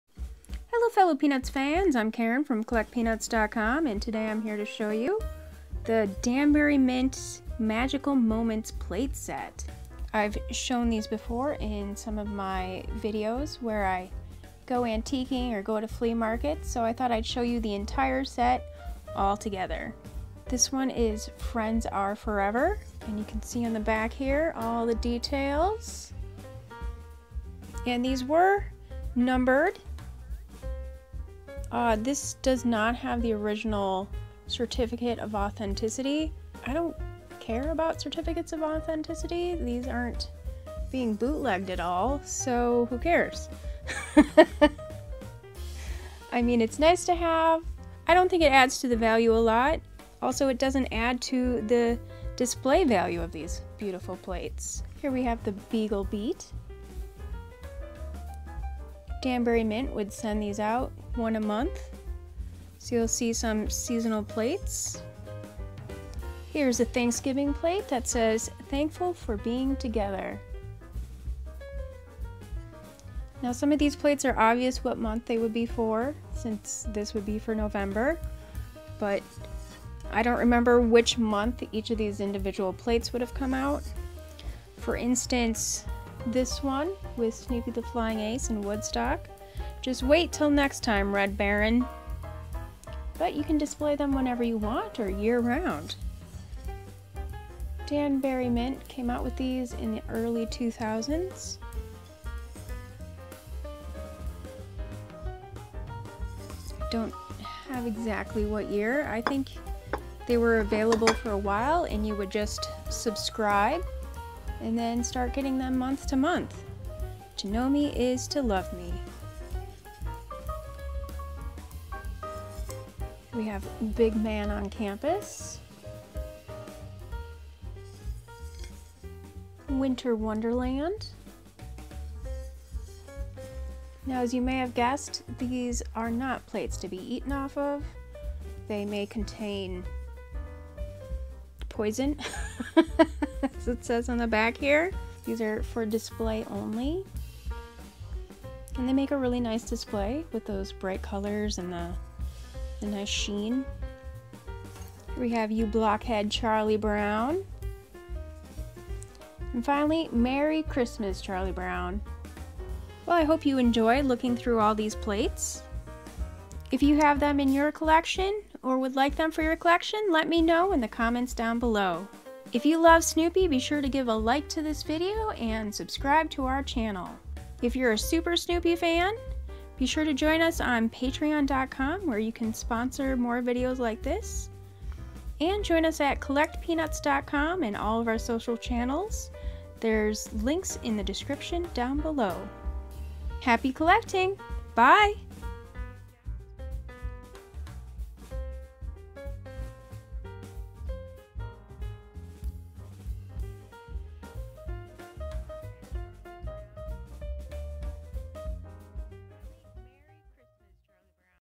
Peanuts Magical Moments Plates by Danbury Mint - Snoopy Find | CollectPeanuts.com
If you've been watching my haul videos, then you may have seen a few of these plates before! Experience the whole set of Peanuts Magical Moments Plates by Danbury Mint, plus learn what I think of "Certificates of Authenticity".

Your feedback lets me know what you want more of!

Title - Peanuts Magical Moments Plates by Danbury Mint - Snoopy Find